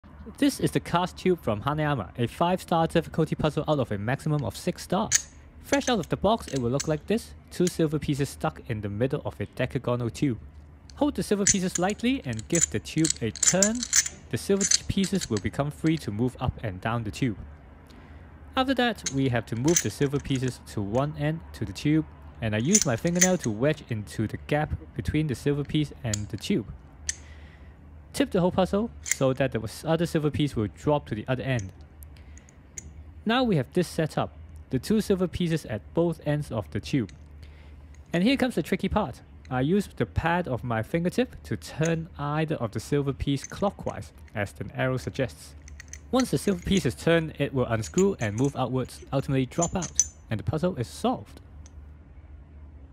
Solve this tube
Hanayama Cast Tube is a relatively new cast metal puzzle.  It is a 5 star difficulty puzzle (out of maximum 6 stars), and it is certainly tricky.  In the video I will show you the solution of this puzzle and how to solve it within 15 seconds!